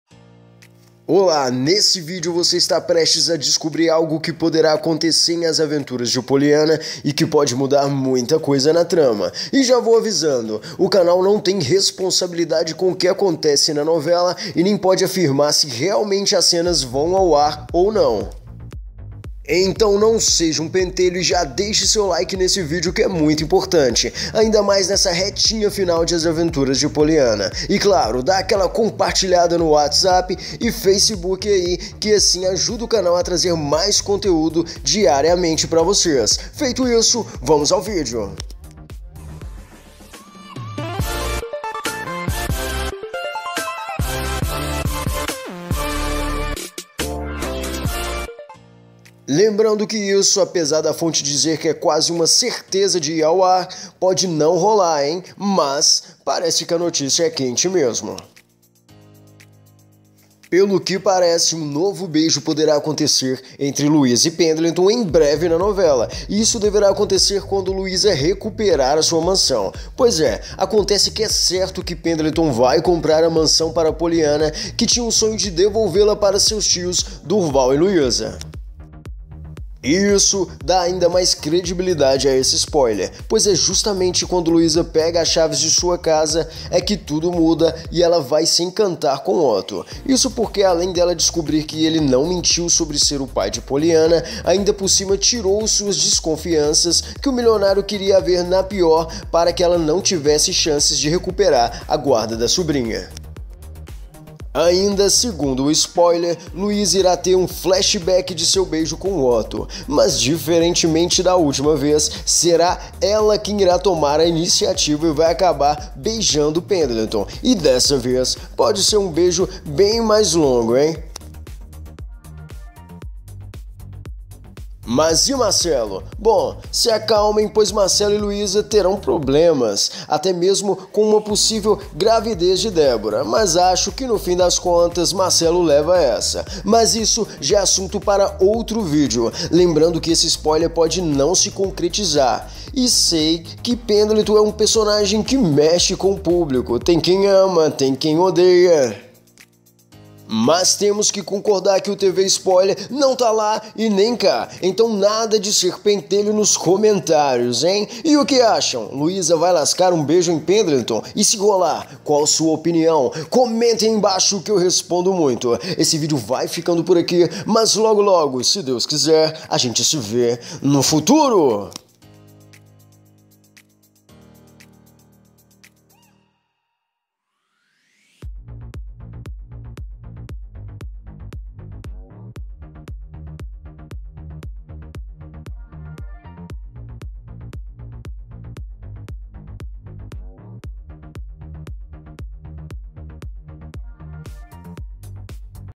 BOMBA! LUISA BEIJA PENDLETON DE NOVO! As Aventuras de Poliana
Luisa poderá beijar Pendleton novamente nos próximos capítulos de As Aventuras de Poliana, e dessa vez a iniciativa poderá ser dela!

Para quem achava que Luotto foi uma coisa do passado, poderá se surpreender nos próximos capítulos de As Aventuras de Poliana.
Pois Luisa pode beijar Otto de novo, e dessa vez a iniciativa partiria dela!

Assista ao vídeo e preste atenção, evite ser agressivo nos comentários!

Esse vídeo fica por aqui, mas se Deus quiser logo logo a gente se vê no futuro!